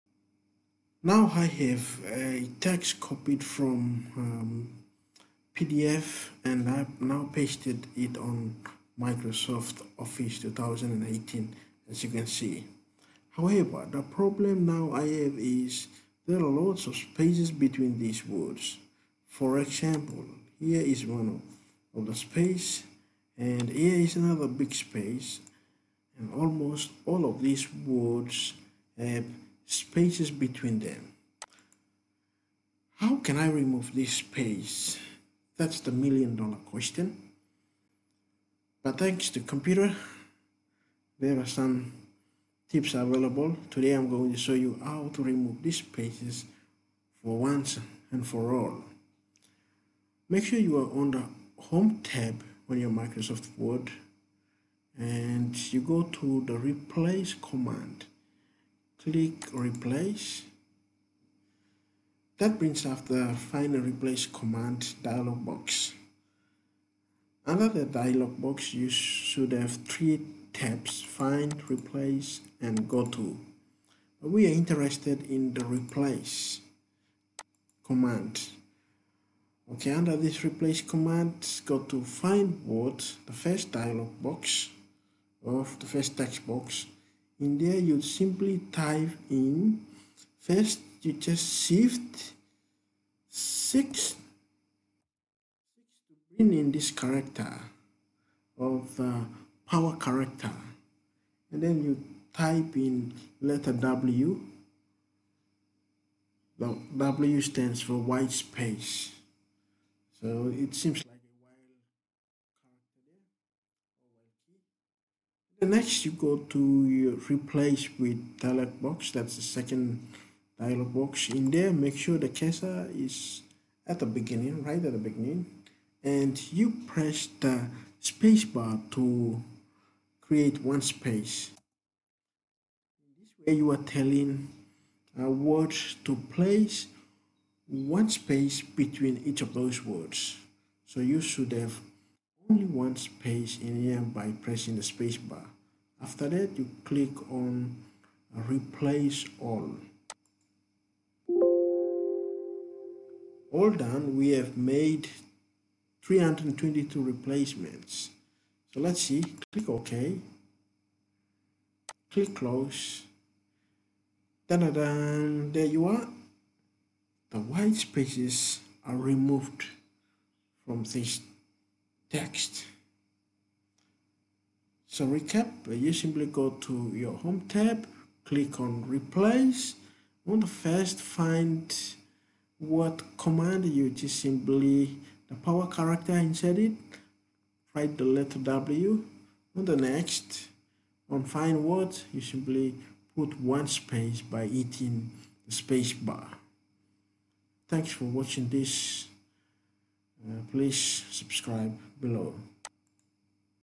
How to remove Spaces between Words from Text Copied from PDF in MS Word
Here is a video on how you can remove spaces between words that are copied from PDF into MS word. Check out our tutorial